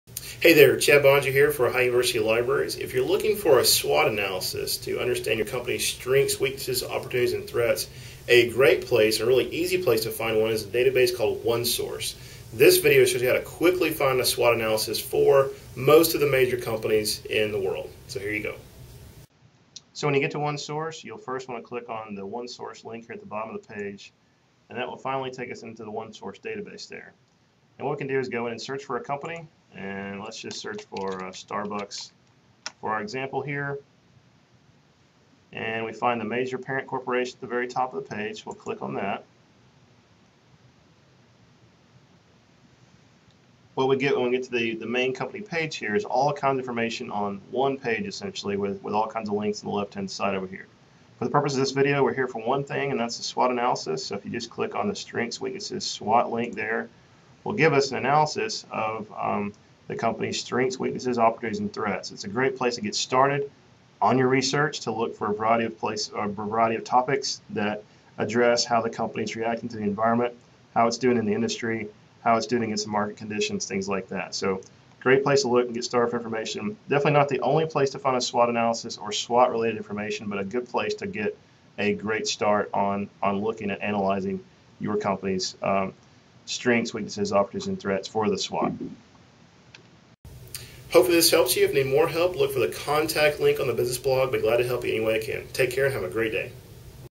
How to find a SWOT analysis in OneSource
If you are looking for a SWOT analysis to understand your company's Strengths, Weaknesses, Opportunities, and Threats, one of the simplest resources to use is OneSource.  This video demonstrates how to quickly find a SWOT analysis for *most* major companies.